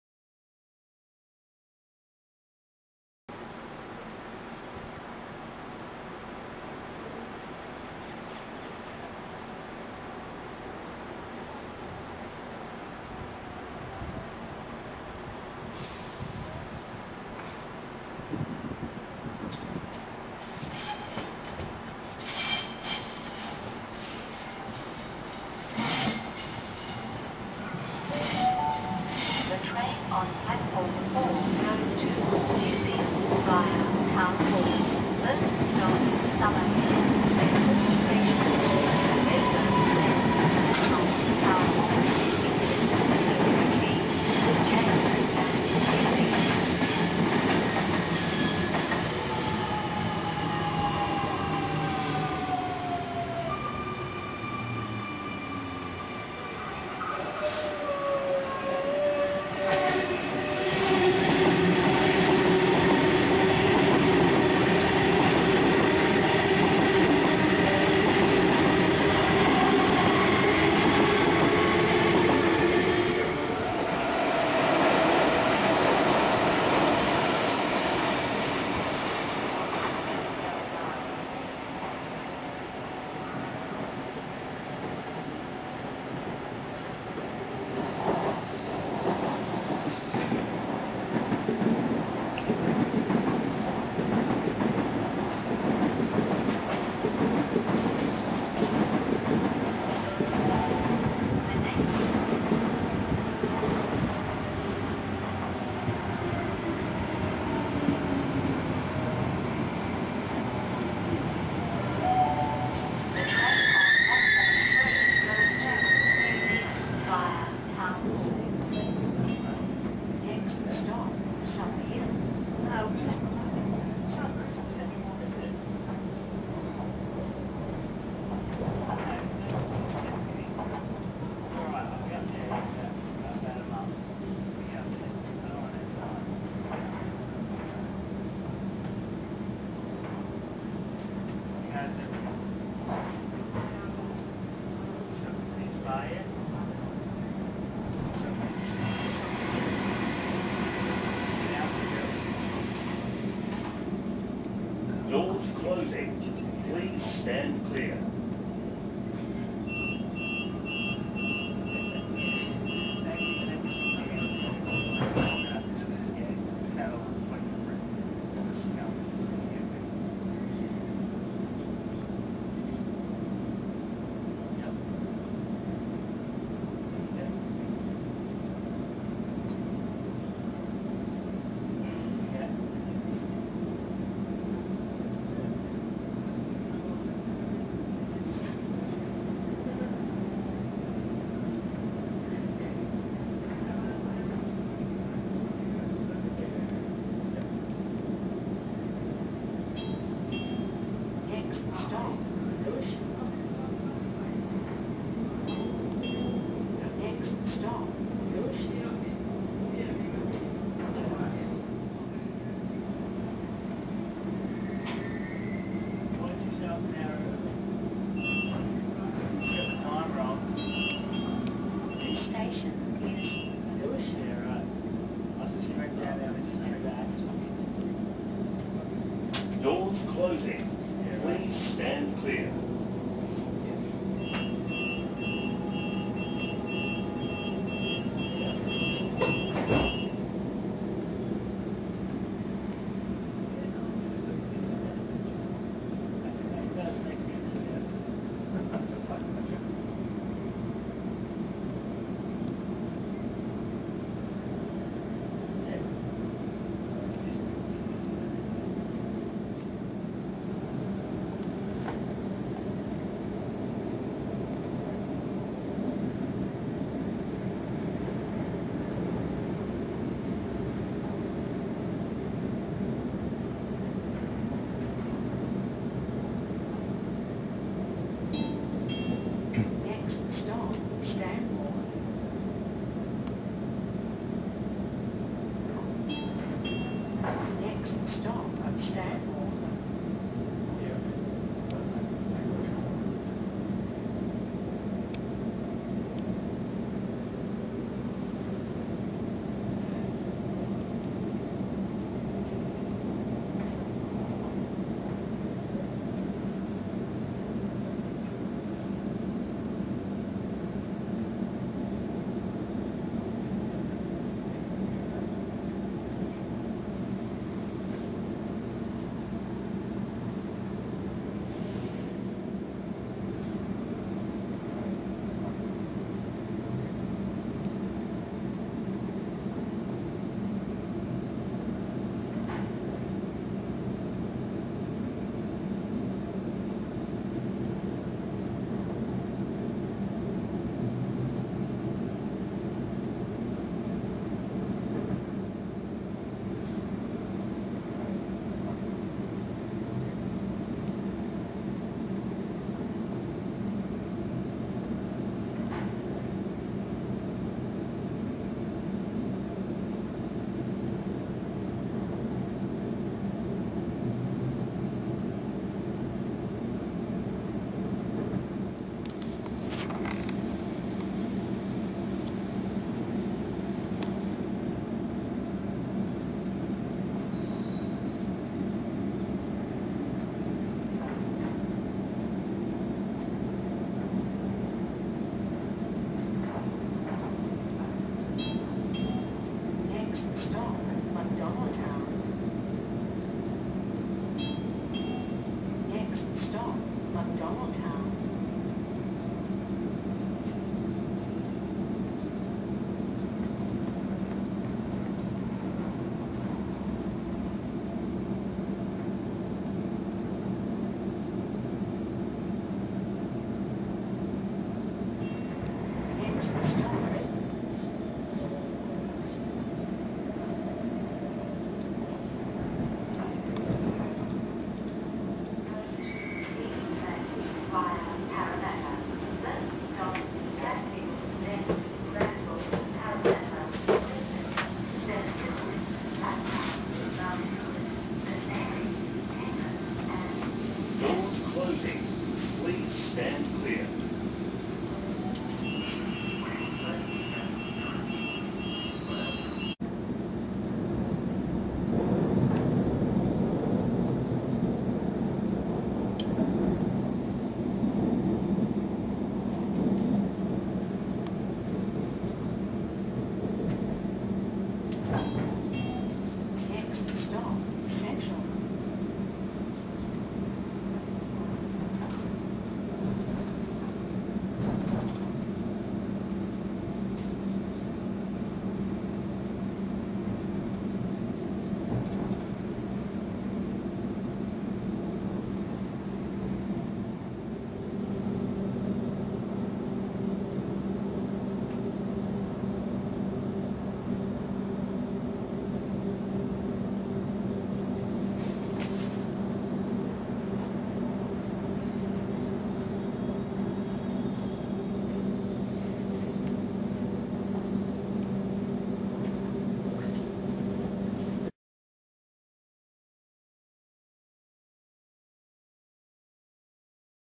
Sydney Train I Ashfield to Central Train Stations Sydney Australia
Sydney Train arriving and departing Ashfield to Central Train Stations Sydney Australia 9/1/13.
5117